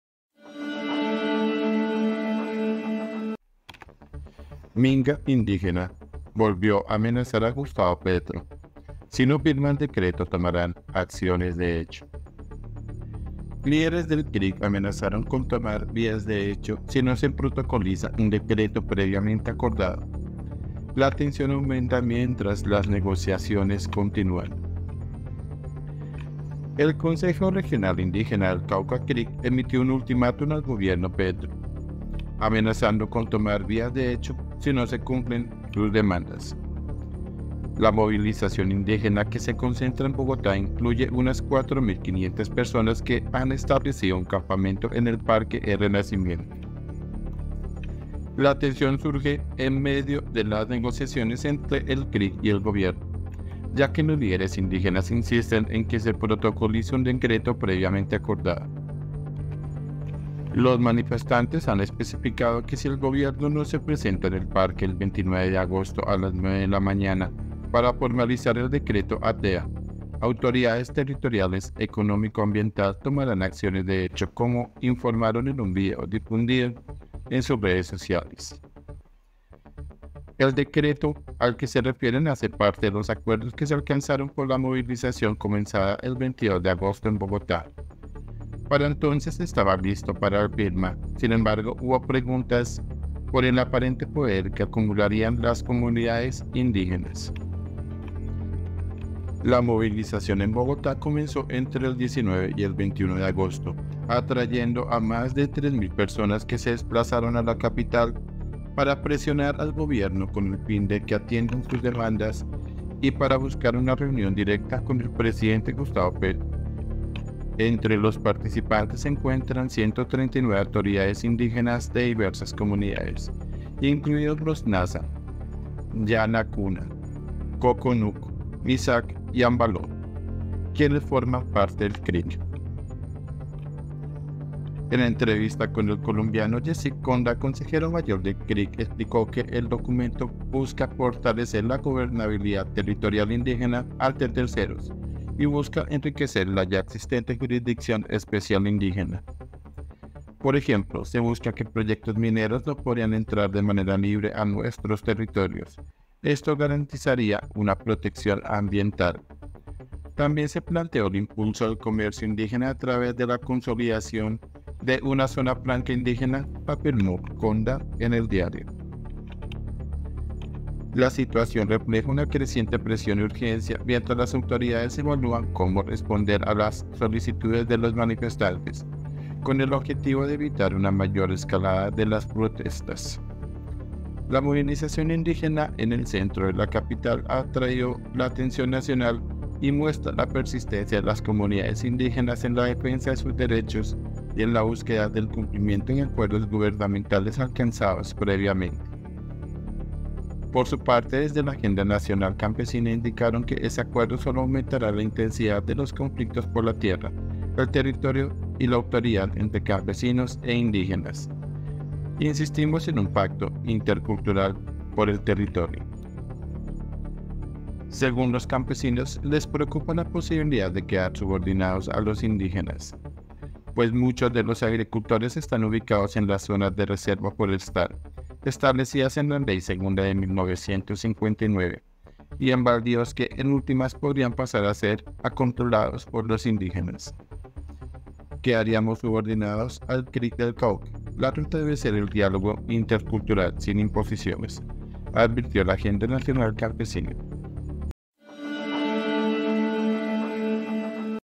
ULTIMÁTUM AL GOBIERNO PETRO 🚨 MINGA INDÍGENA “TOMARÁN ACCIONES DE HECHO”
Minga indígena volvió a amenazar a Gustavo Petro: si no firma el decreto “tomarán acciones de hecho”
Líderes del Cric amenazaron con tomar “vías de hecho” si no se protocoliza un decreto previamente acordado. La tensión aumenta mientras las negociaciones continúan
INFOBAE